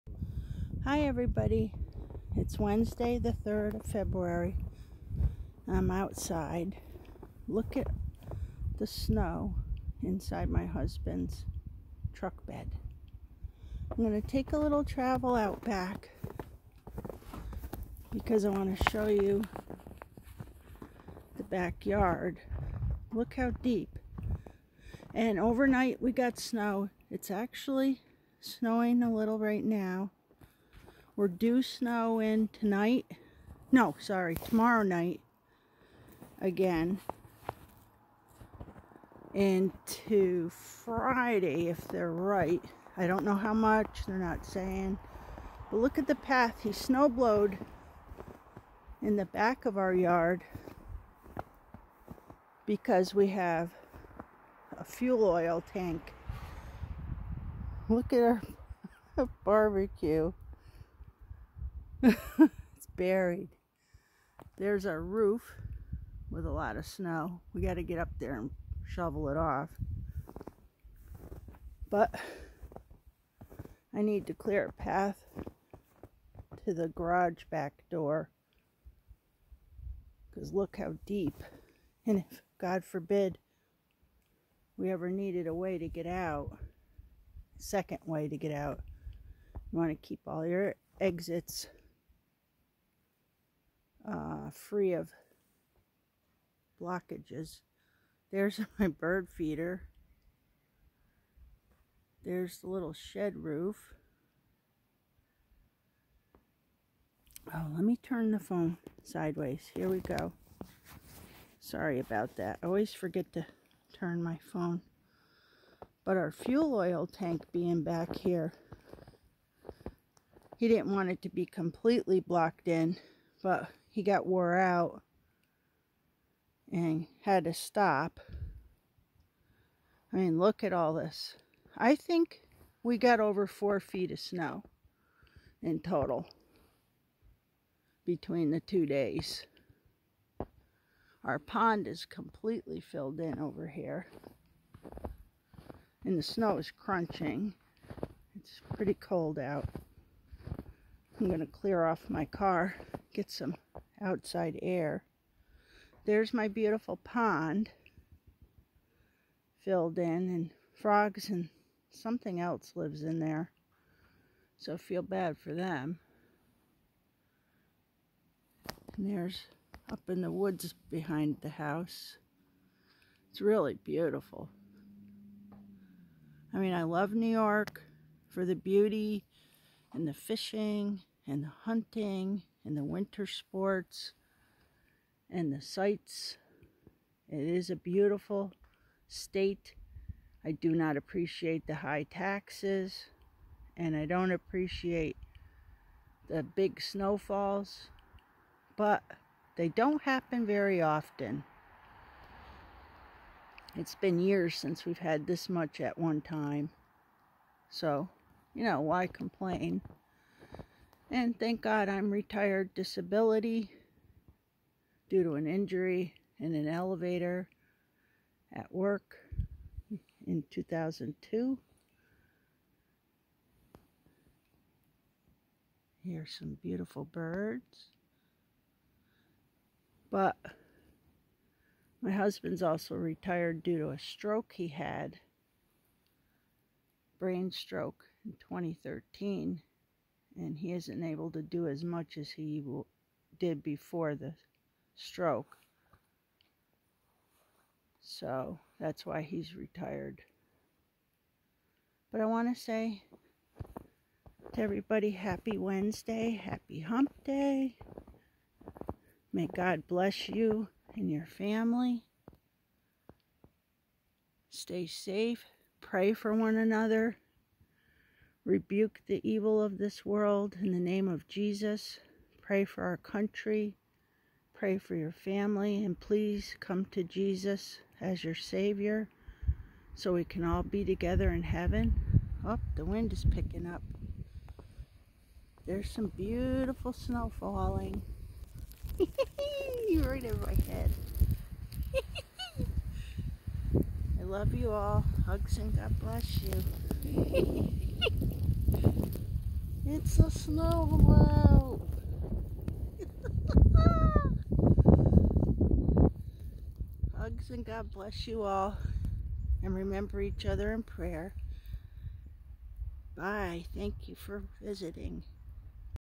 snowstorm aftermath February 3 2021 New York state by Pennsylvania border
Got outside to clean off my car and walk around the yard to see 👀 the snowstorm aftermath.  HUGS and GOD bless you all 🤗.